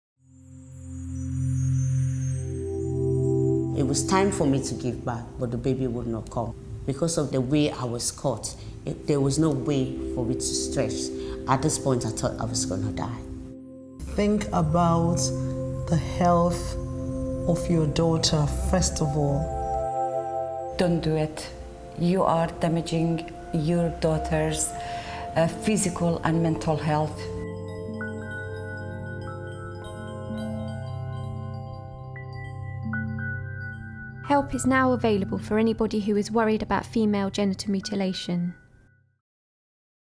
Community speaks out against
Together we can raise awareness of female genital mutilation (FGM) and encourage people to report it. FGM is illegal. Let's protect our girls.